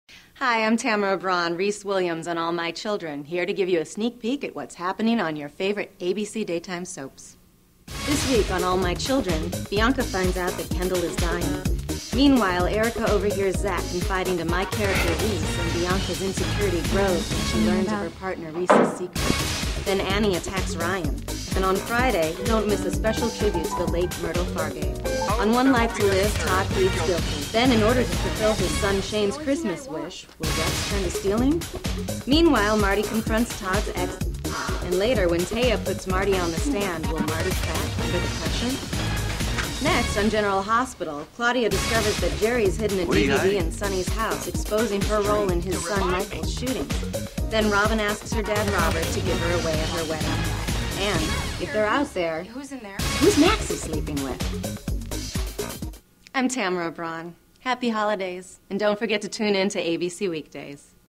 Soap Scoop 15-22/12/2008
AMC- Bianca/Reese, Erica snoops/Zach, Myrtle remembered, Annie/Ryan
OLTL- Todd pleads Guilty, Rex/Shane, Tea/Marty
GH- Robin/Robert, Claudia/Sonny, Maxie/Spin/Lulu/Johnny